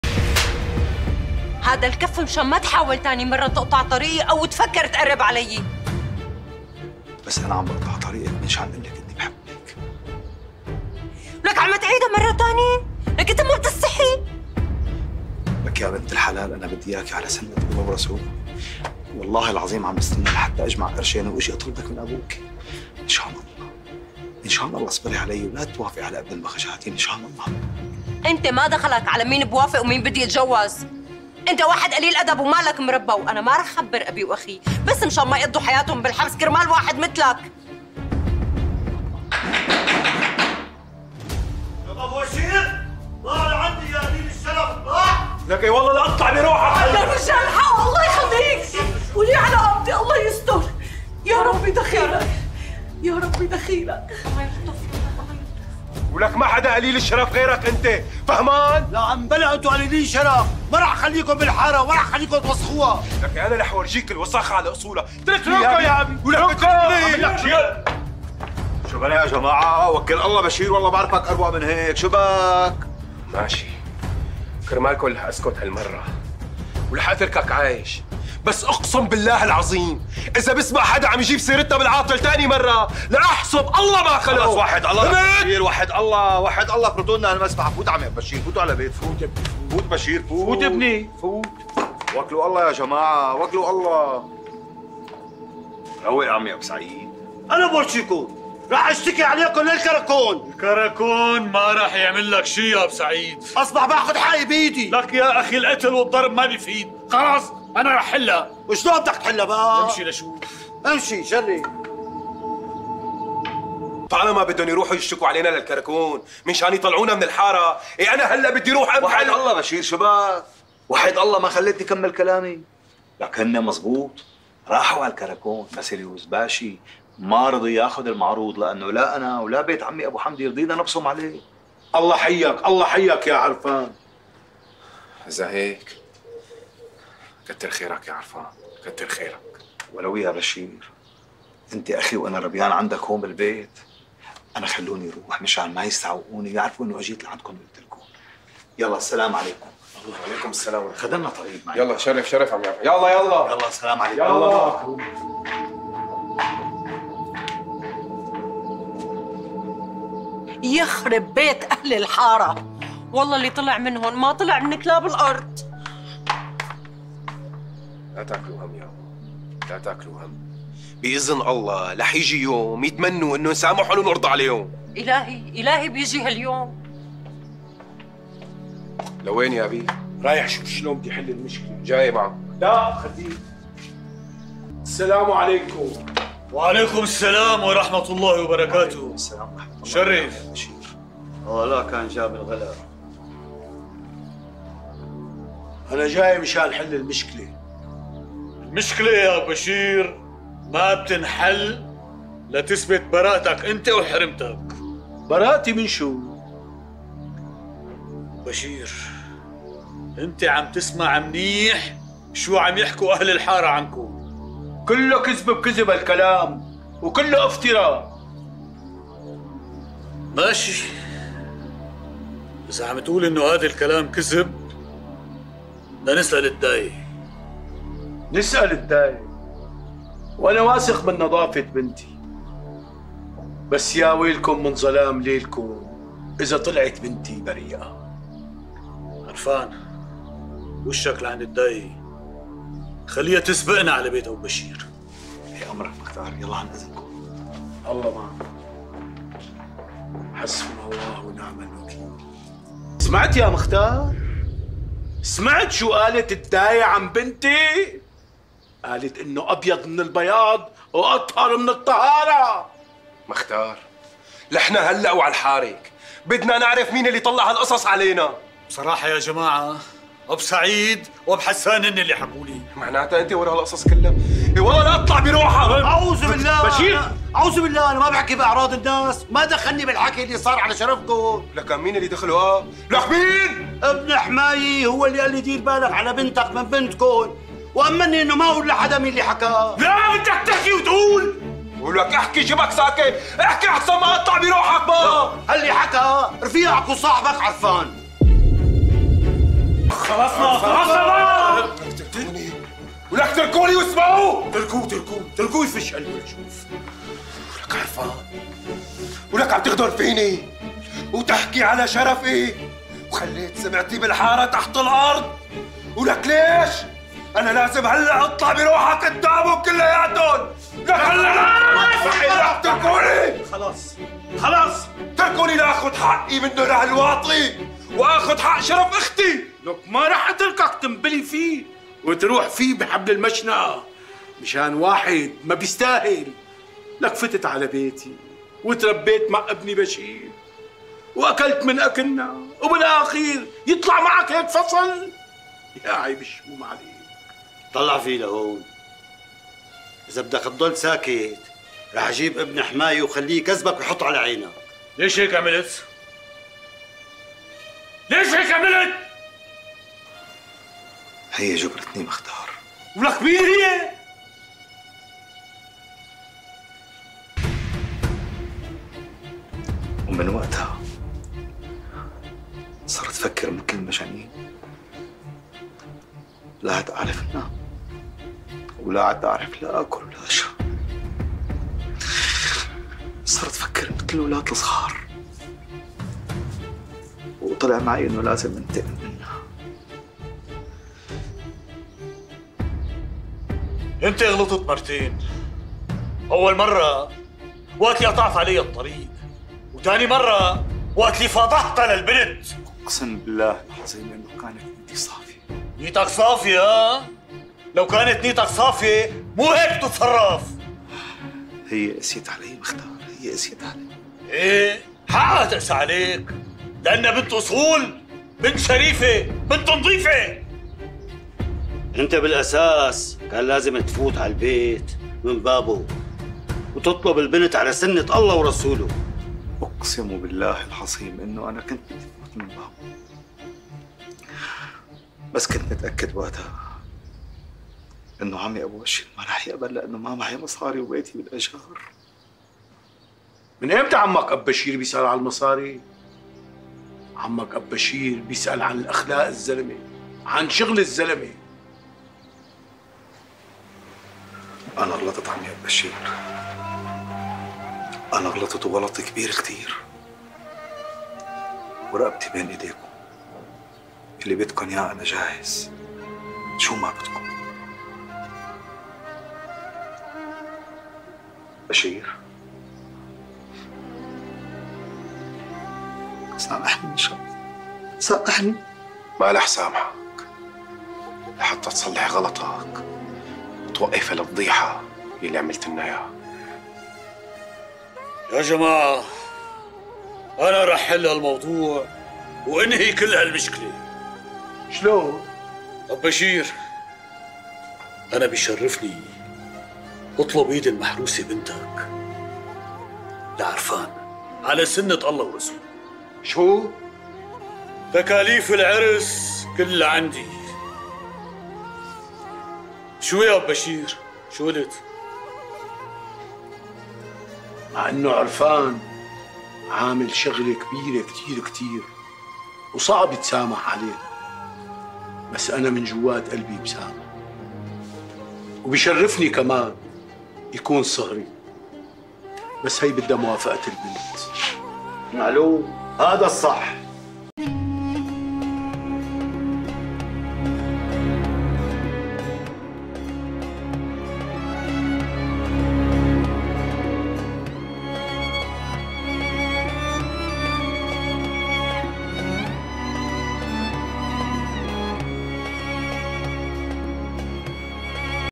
رفضـته لأنه فقير قام فضـحها هي واهلها بالحارة لتطلع ريحتها وياخدها غصب عنها
من روايات